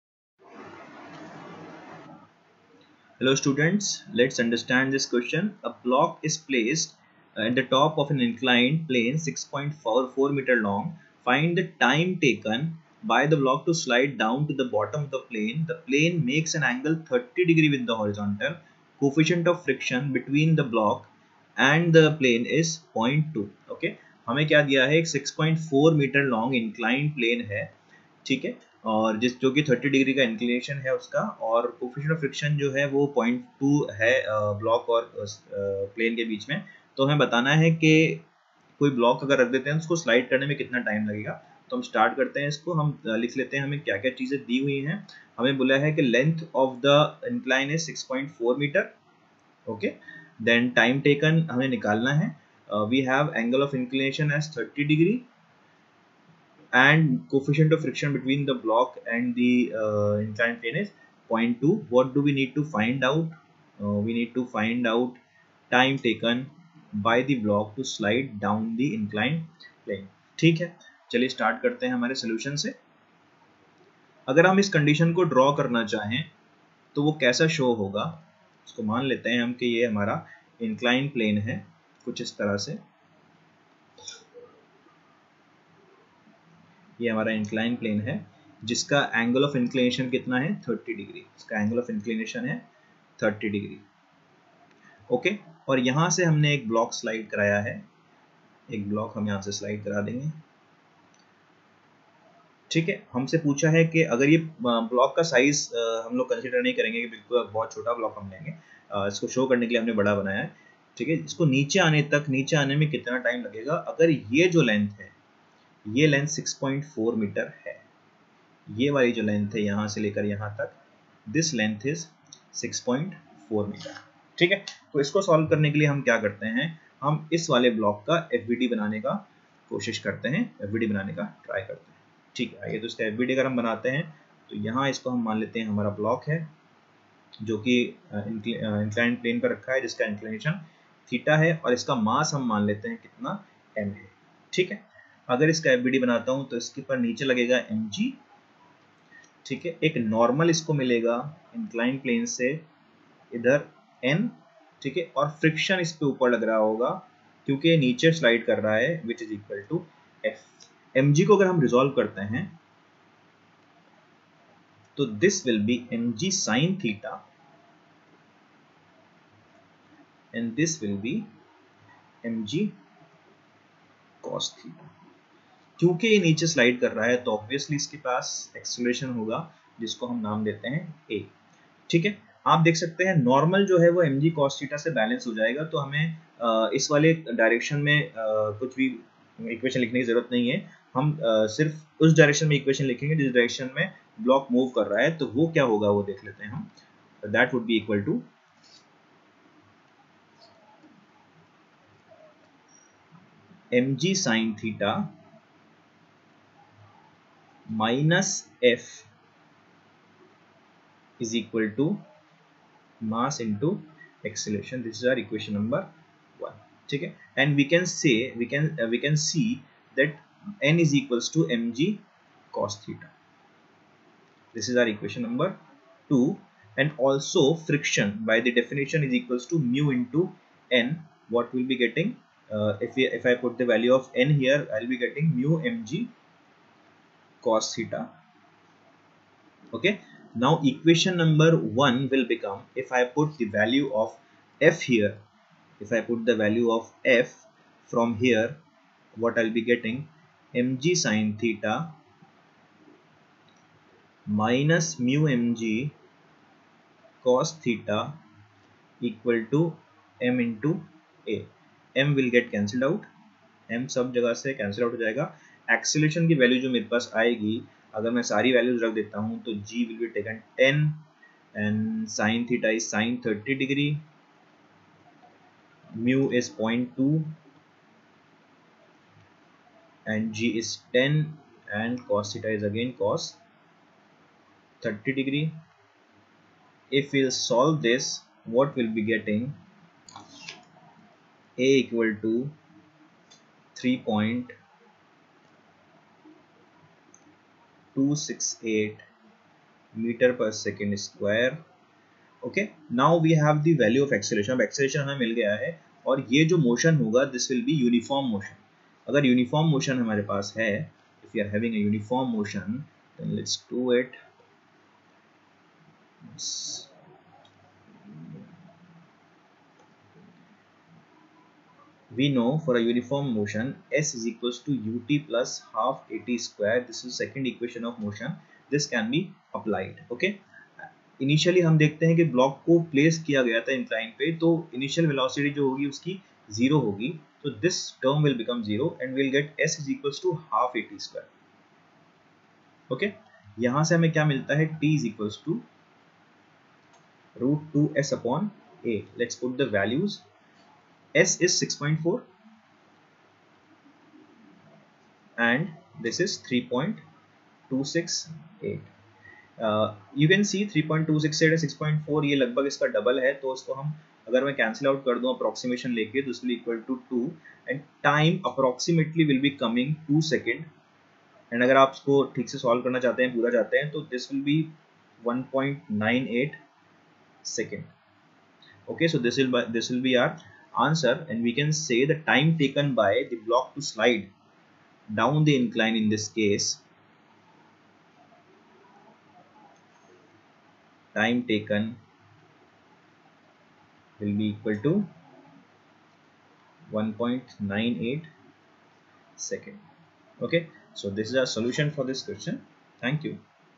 A block is gently placed at the top of an inclined plane 6• 4m long. Find the time taken
A block is gently placed at the top of an inclined plane 6• 4m long. Find the time taken by the block to slide down to the bottom of the plane. The plane makes an angle 〖30〗^∘ with the horizontal. Coefficient of friction between the block and the plane is 0⋅2 . Take g=10m/s^2